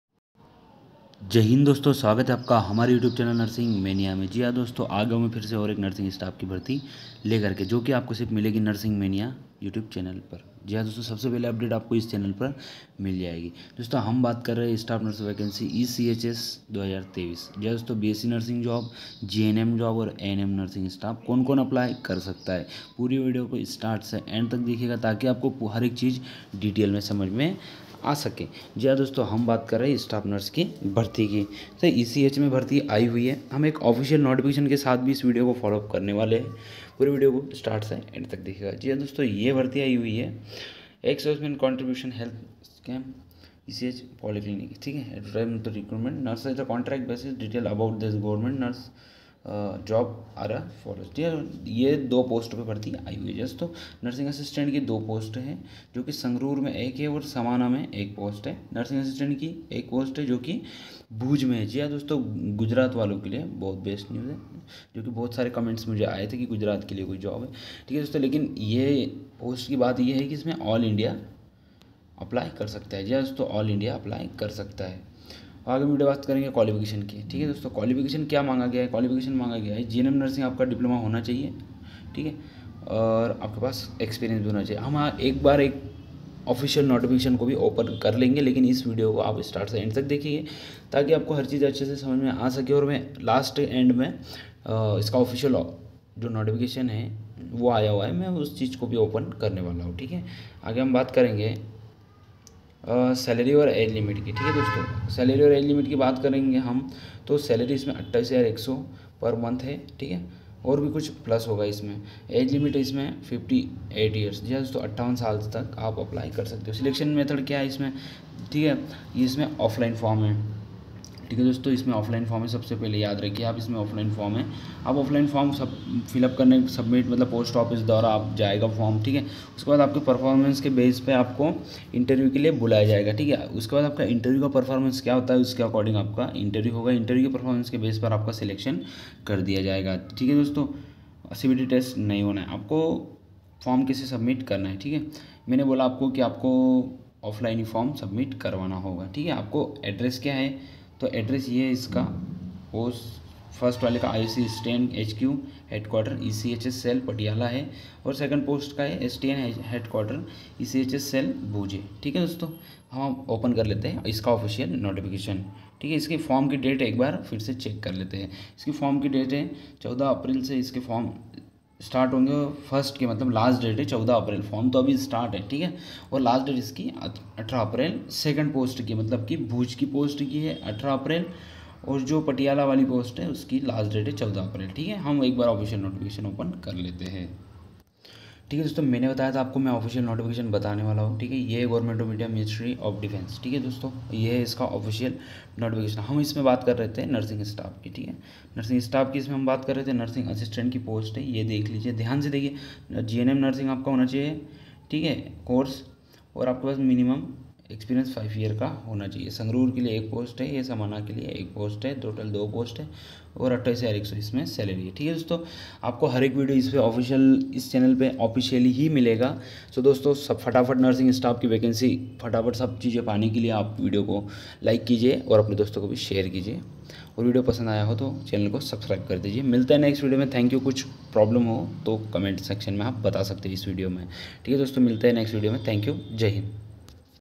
ECHS New Nursing Staff Recruitment||Defence Peramedical Staff Nurse Vacancy Notification Out
ECHS Staff Nurse Recruitment 2023
Last date-14/04/23
Age limit -58 yrs
A- Patiala
B- Bhuj (Gujrat)
Please Support to my channel 👇
This video are following the all you tube and Google guidelines, Turm and conditions.